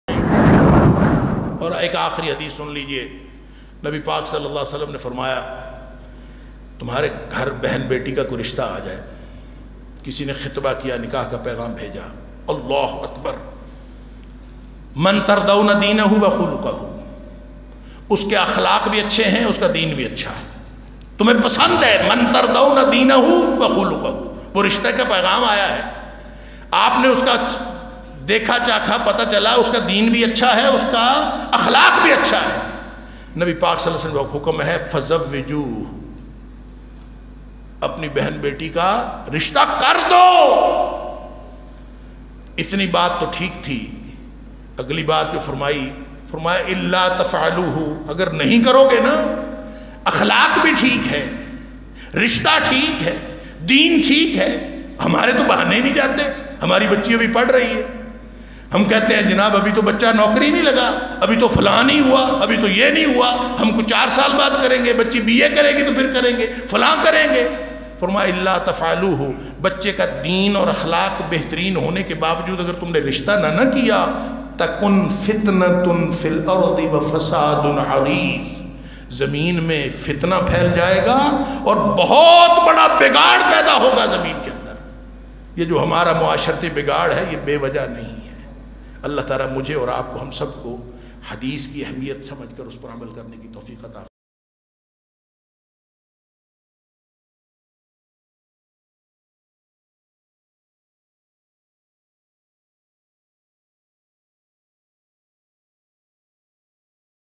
behn beti ka rishta aaye to ye galti kabhi na karna | beti ka rishta karte waqt | dr hammad lakhvi
iss video mai dr hammad lakhwi sahab nai intihai khubsurat or apny makhsoos andaz mai logon ko bataya ky behn beti ka rishta aaye to ye galti kabhi na karna aakhir ye konsi galti hai dekhye is video mai, bohat hi piyara bayan aap khud bhi dekhye or sunye or aagy bhi apney doston bhaiyon tak ye pegham pohchaiye jazakallah,

Most populer videos links,